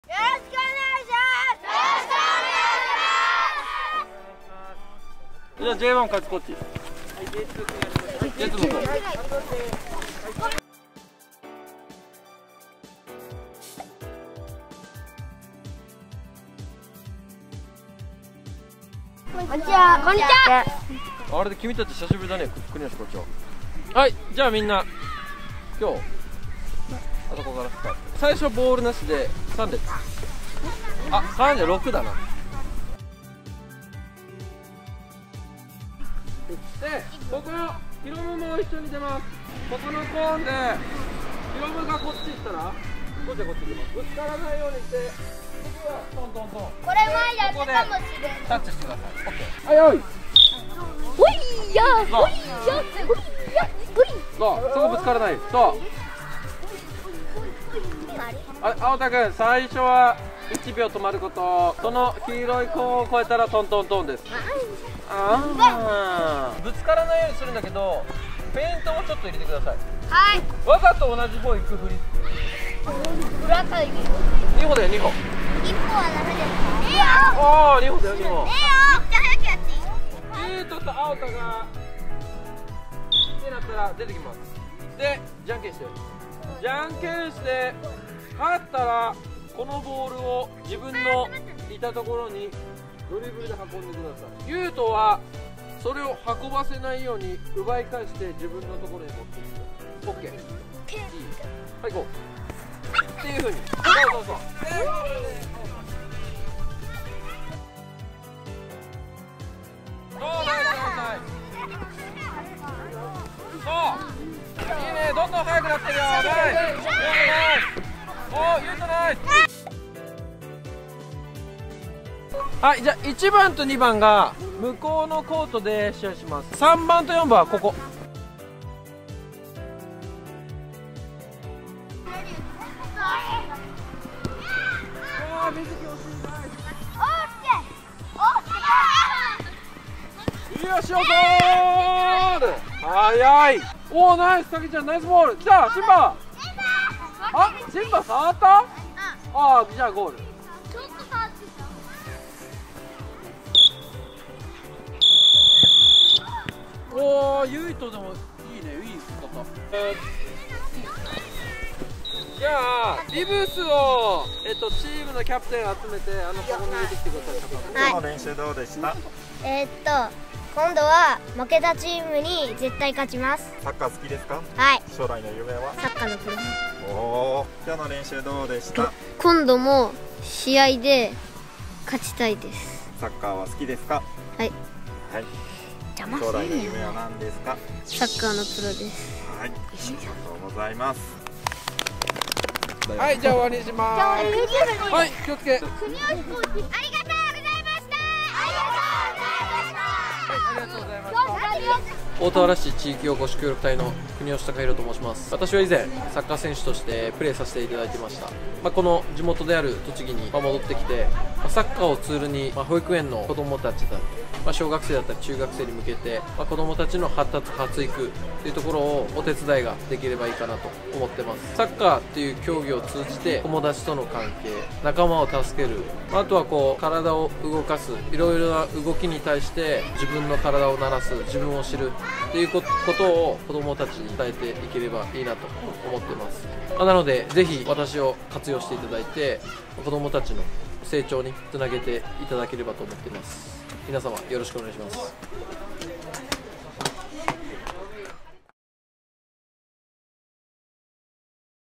【大田原市地域おこし協力隊】國吉 貴博さんの指導風景
大田原市地域おこし協力隊の國吉 貴博さんのご紹介です！

埼玉県出身、那須塩原で育った元プロサッカー選手。
現在は、大田原市地域おこし協力隊として、小・中学生・高校生を中心に多くの子供たちにサッカーを教えています。

今後は保育園・幼稚園等の園児に向けてもサッカーを通じて体の動かし方を伝えていきます。

大田原市政策推進課